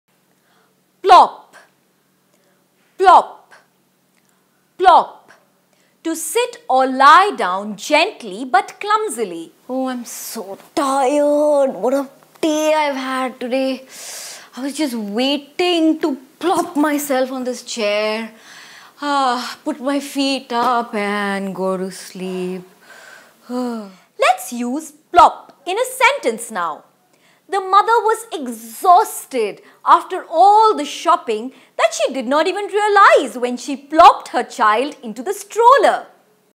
Meaning of Plop
PLOP
MEANING: sit or lie down gently but clumsily.
SENTENCE: he plopped himself down on the nearest chair.